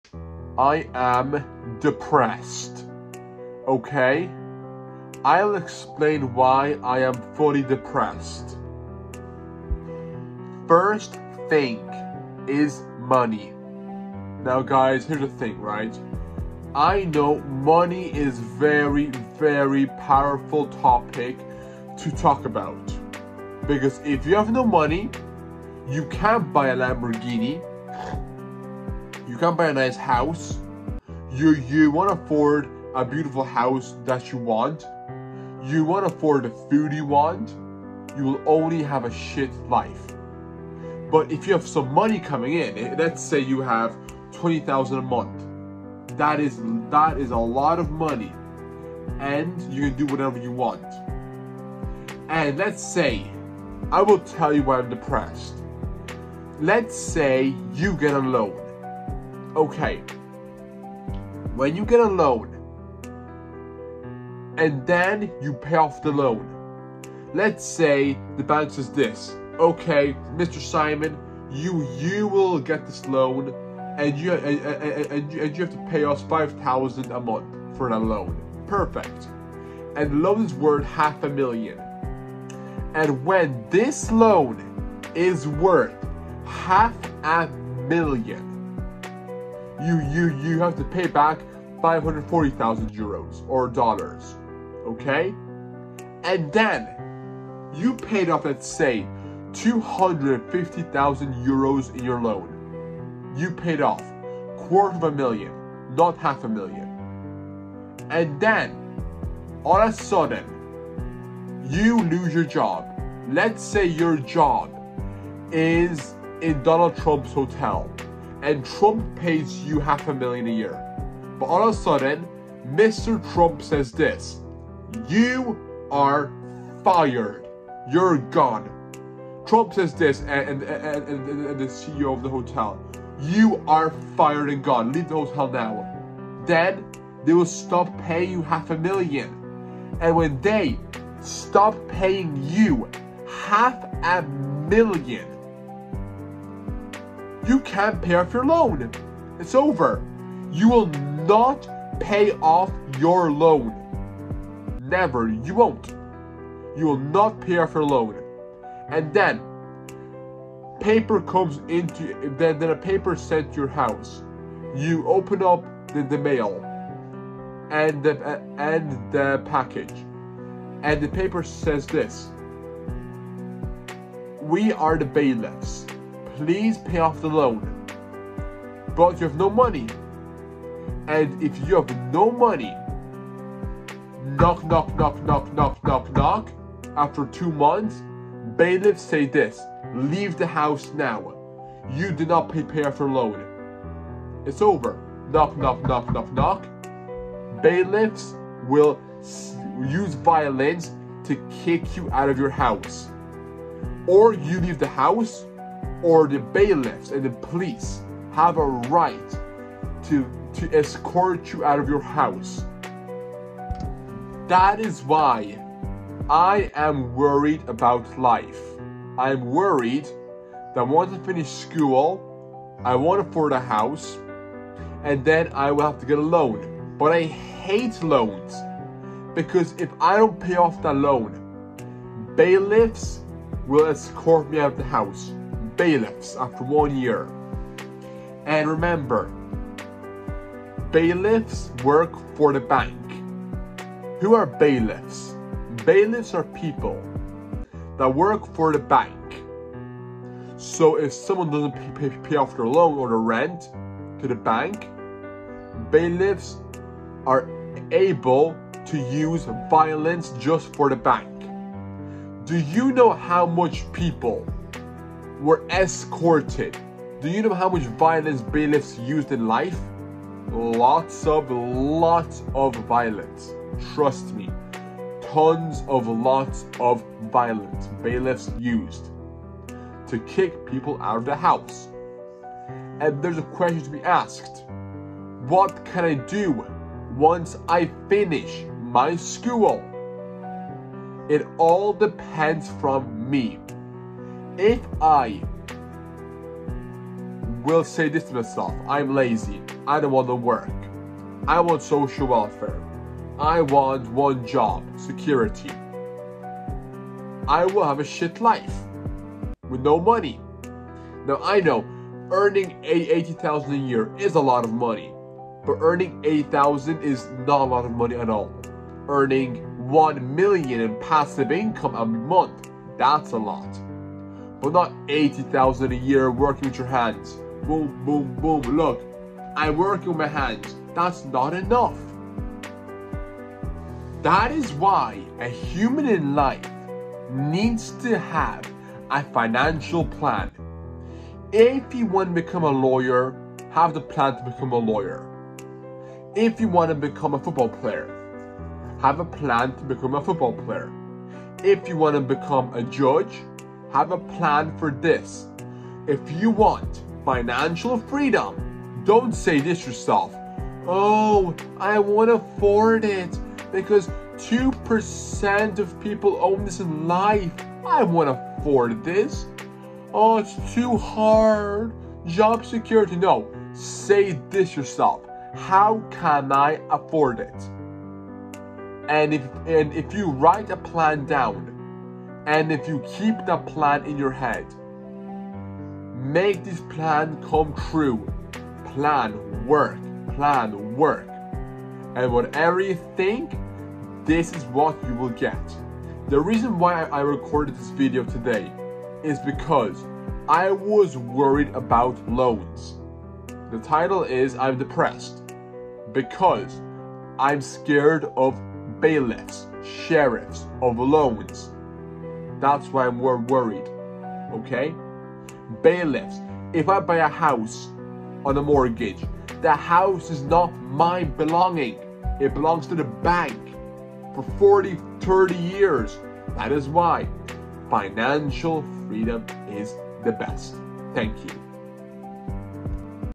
I’am depressed from loans
I used the title I am depressed because someone in my school was talking about loans and bailiffs ￼and I was worried that they might take my house away in the future so I hate loans that I why I used the title I’am depressed